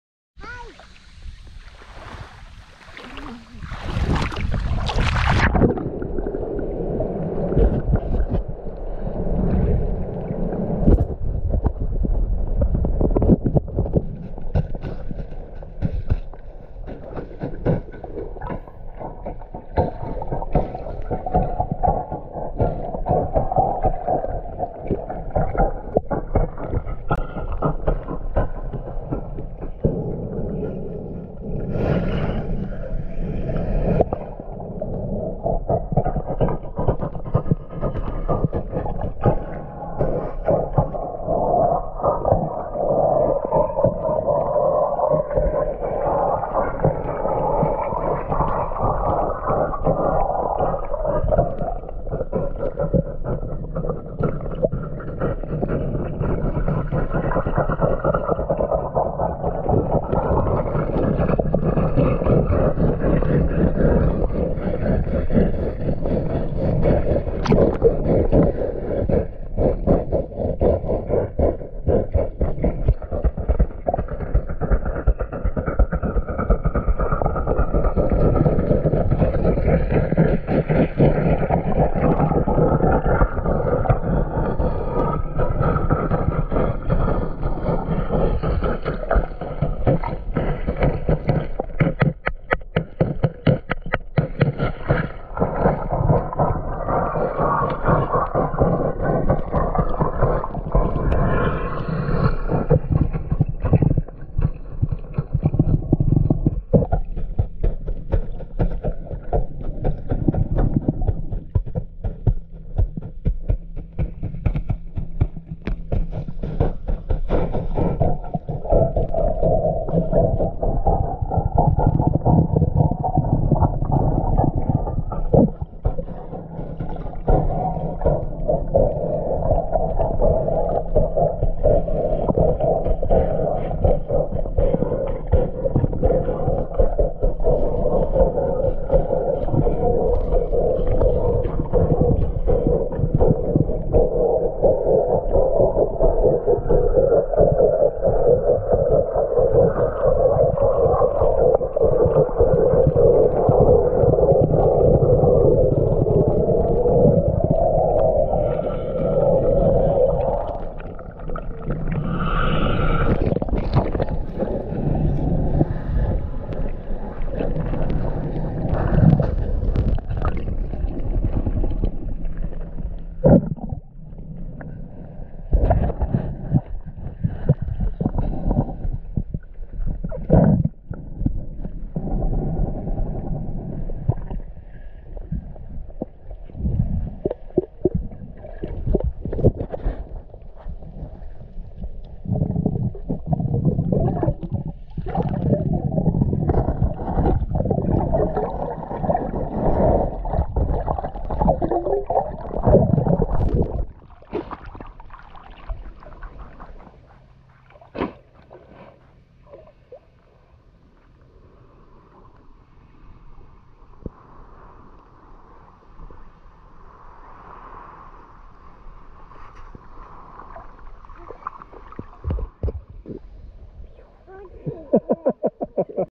earth dance family / 地球ダンス家族 1 志摩 宮の浦- 坪内一家
志摩の海で海の底を叩いた音と、水中ダンス。

出演
菜央  / 水中ダンス
坪内あつし / 海底
まあや / ダンス